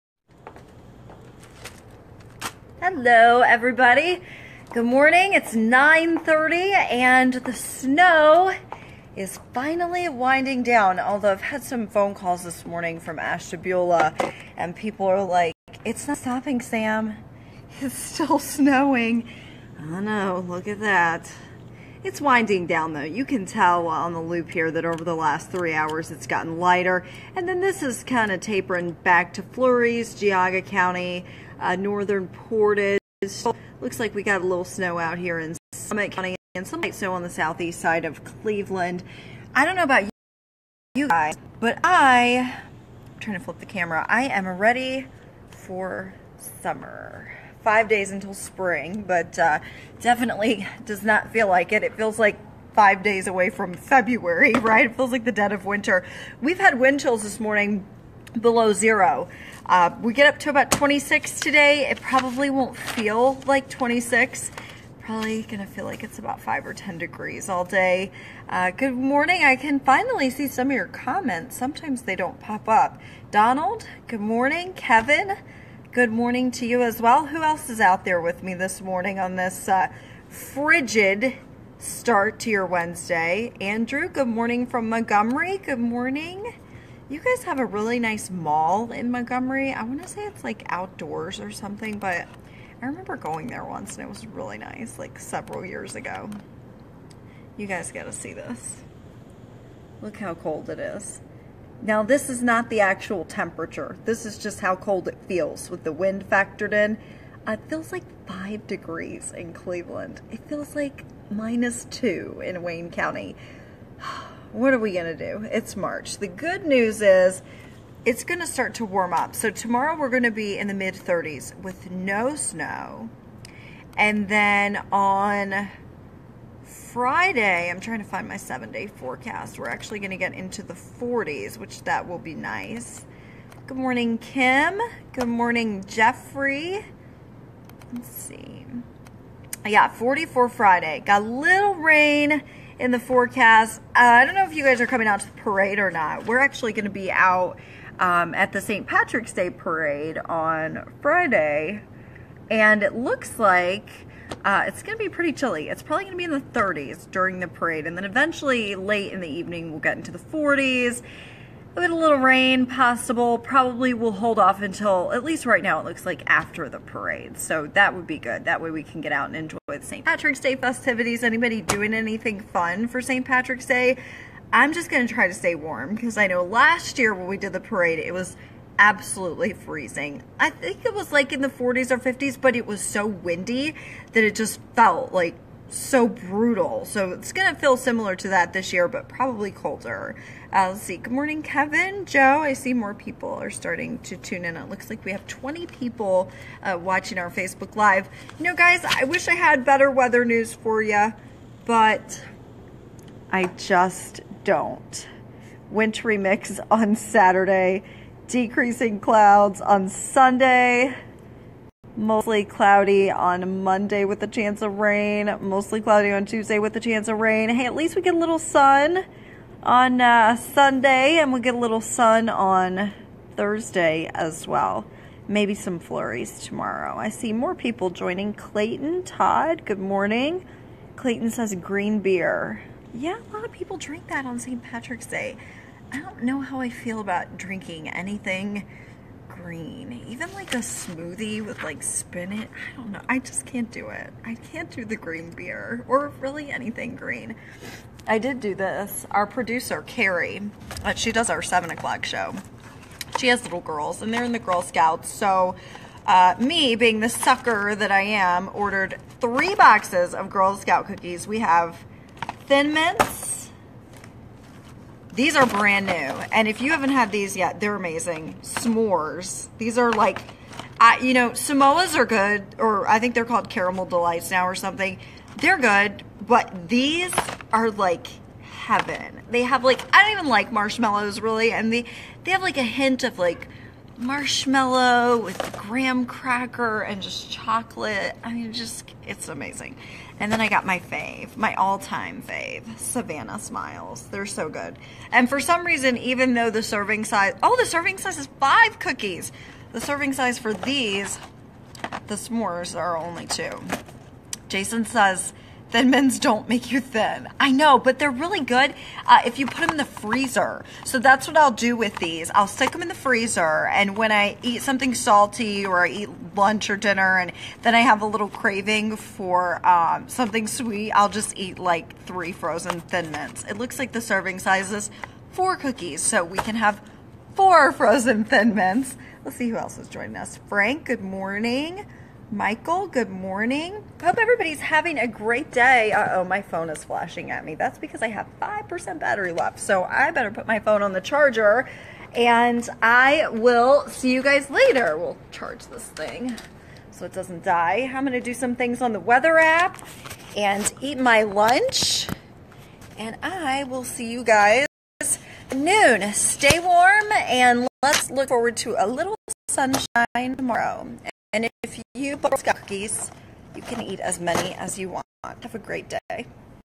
5 DAYS TIL SPRING!
Meteorologist Samantha Roberts WOIO CBS19 Cleveland Ohio Weather For Wednesday Morning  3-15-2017